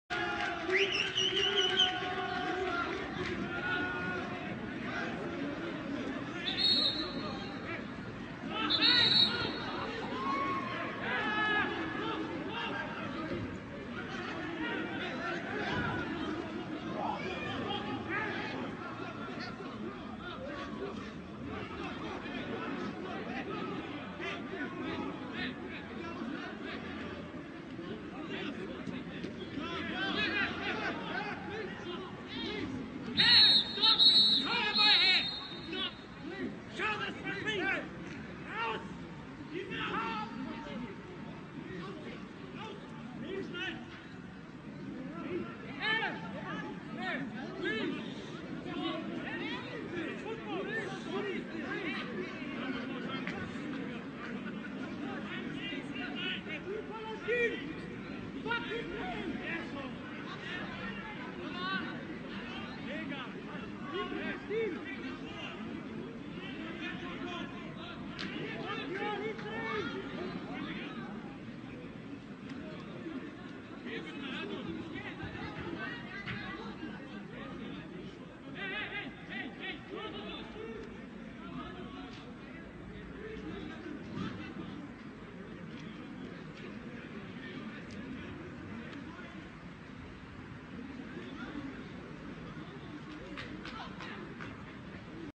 جماهير تعتدي على لاعبين صهاينة تضامنا مع غزة مباراة بين فريقي ليل الفرنسي ومكابي حيفا الصهيوني
م إيقاف مباراة ليل الفرنسي و ماكابي حيفا الصهيوني على إثر تدخل جمهور فرنسي و عربي متضامن مع غزة و يرفع الرايات و الأعلام الفلسطينية و يطرد و يضرب لاعبي الفريق الصهيوني